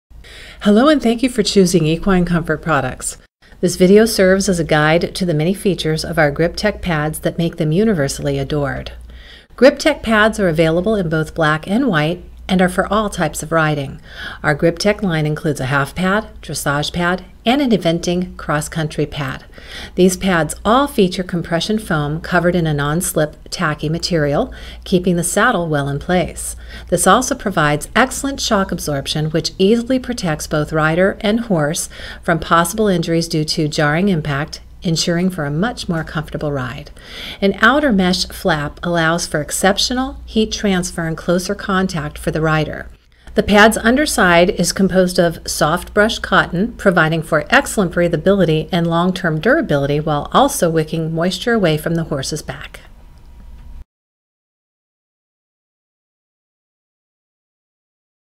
Equine Comfort Products ECP Grip Tech Pad
This video serves as a guide to the many features that make ECP 's Grip Tech Pads a premier choice for all types of riding.